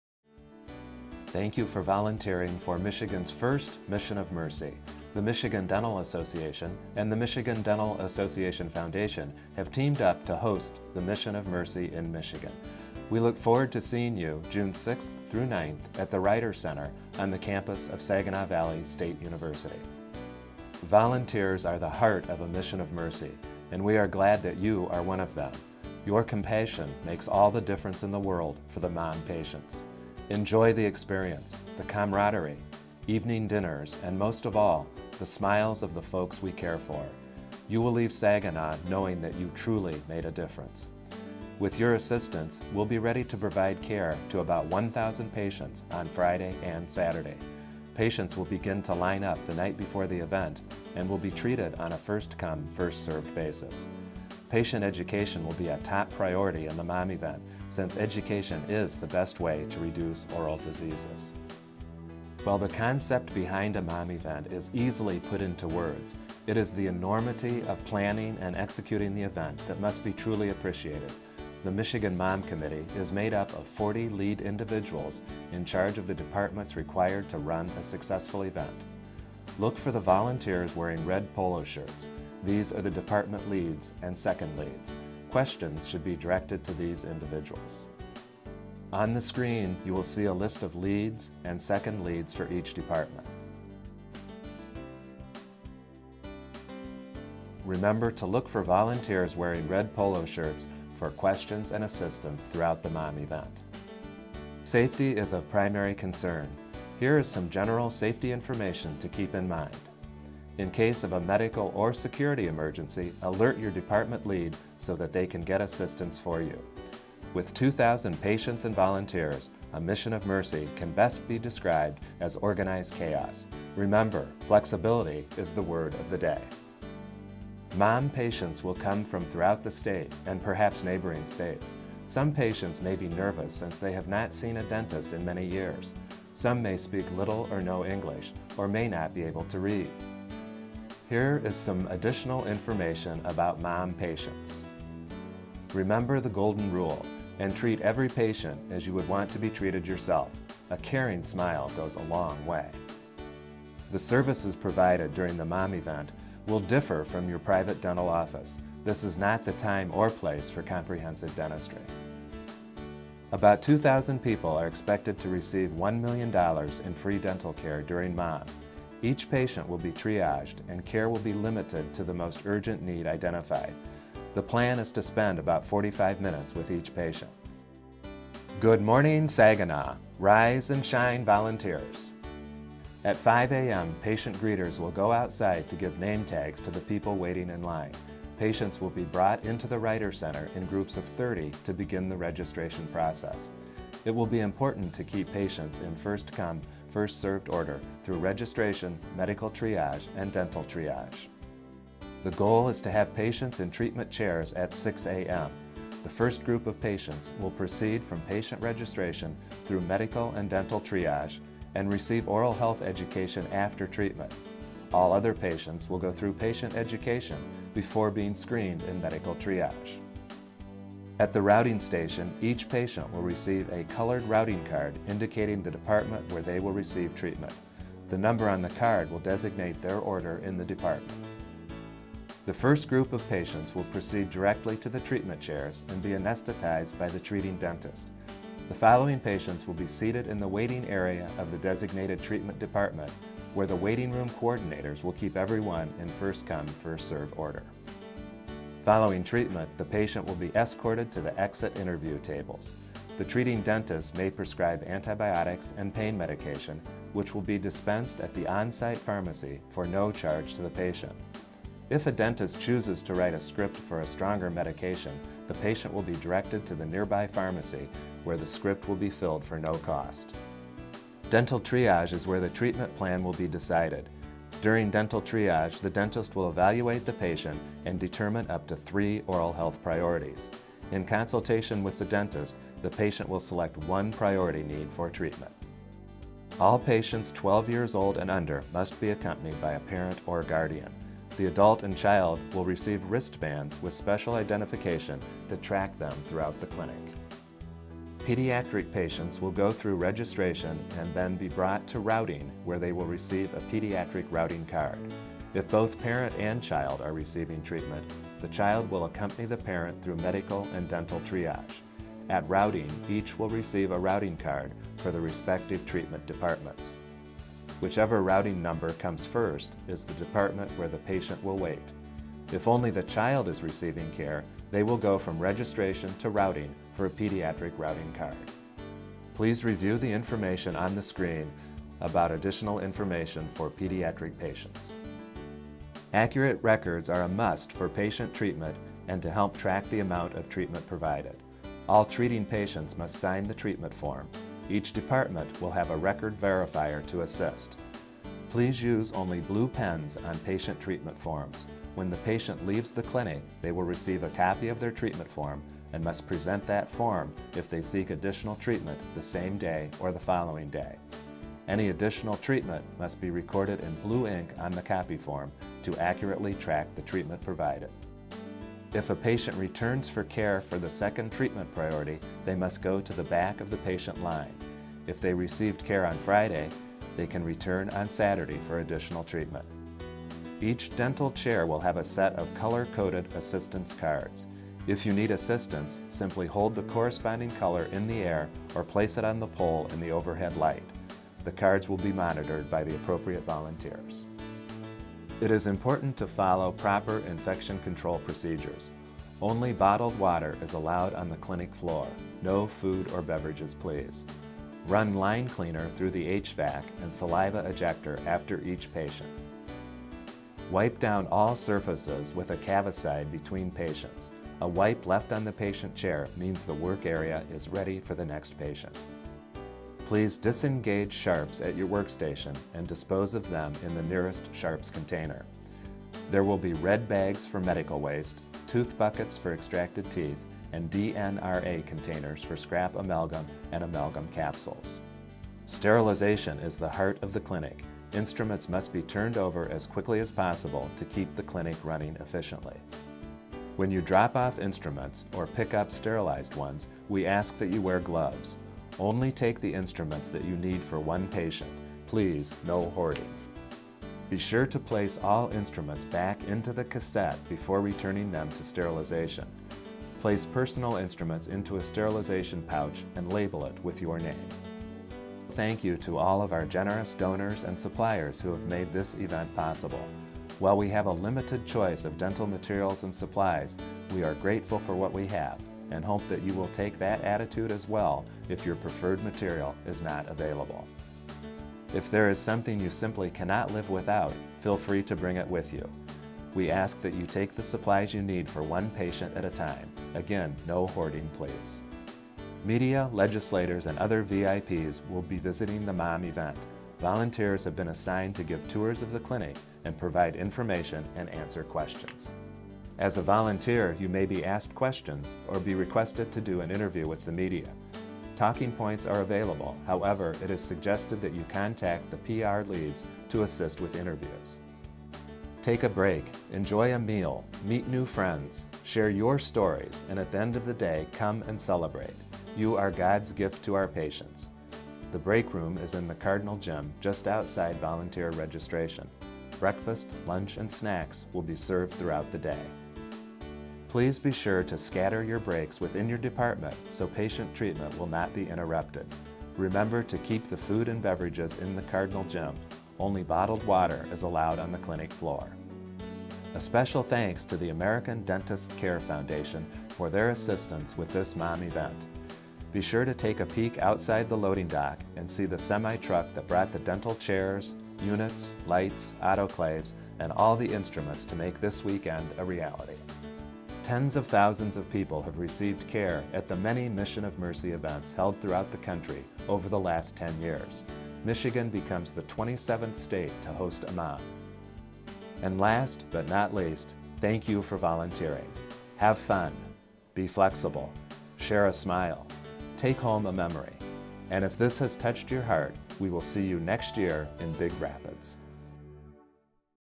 Mission of Mercy - Volunteer Orientation
Thank you for volunteering for Michigan's first Mission of Mercy event!

This brief tutorial will tell you everything you need to know to arrive in Saginaw and make an impact.